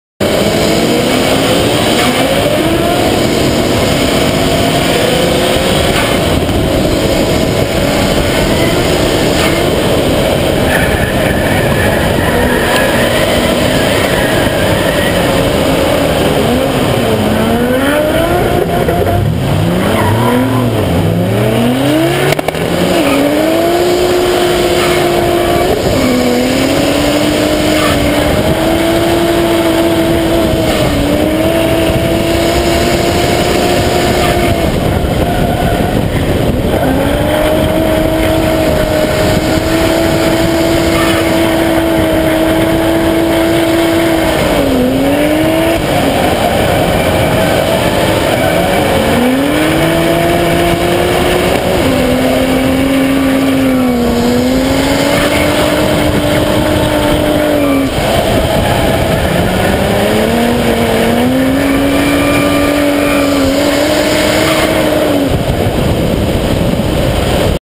Mitch Chasing @ Drift Wa round 2
Shot @ Barbagallo Raceway in Perth Western Australia using a GoPRO HD Camera.